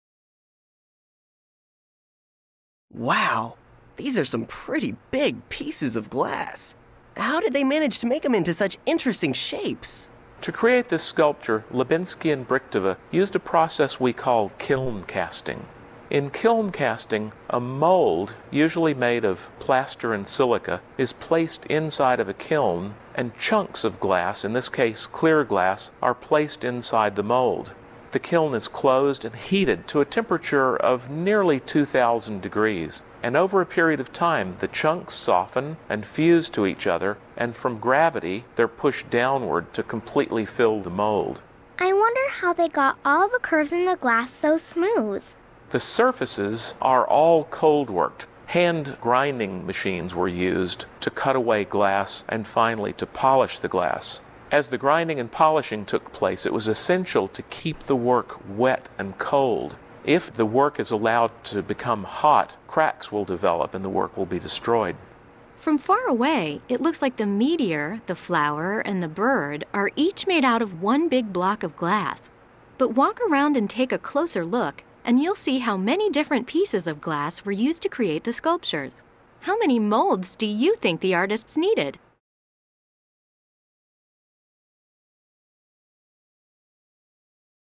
Libenský Installation (Technique)
Listen as glass artist William Gudenrath, describes the techniques used to create "Meteor, Flower, Bird" by Czech artists Stanislav Libenský and Jaroslava Brychtová. The artists convey three messages with this sculpture. Meteor, on the left, represents Corning as an international center for the study of glass. The flower, in the middle, represents the beauty of glass. The bird says that working with glass, like bird song, is a common language spoken around the world.